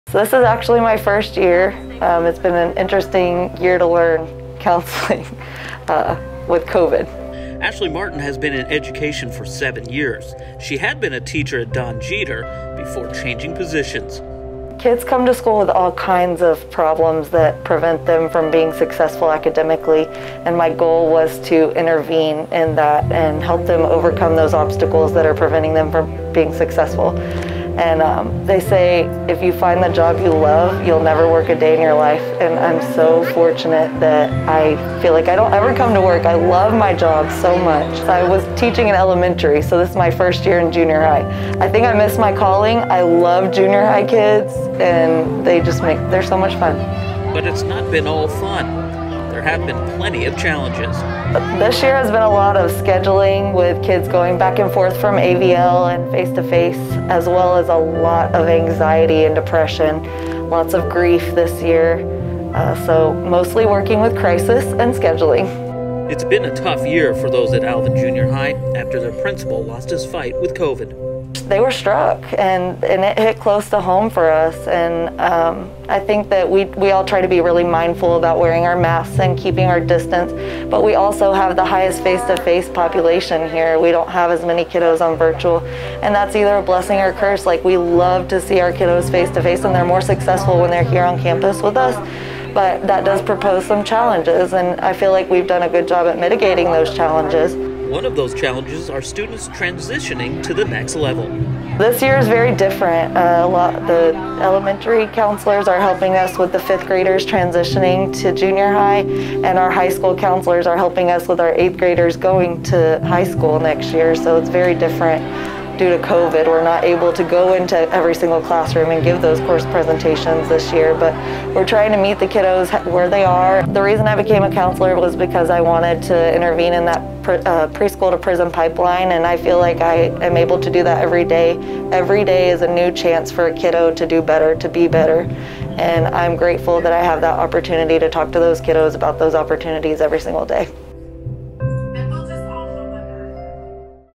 School Counseling at the Junior High Level
Ashley Martin is a new school counselor at Alvin Junior High School and we see how she's liked the transition from being an elementary school teacher. It's all part of National School Counseling Week.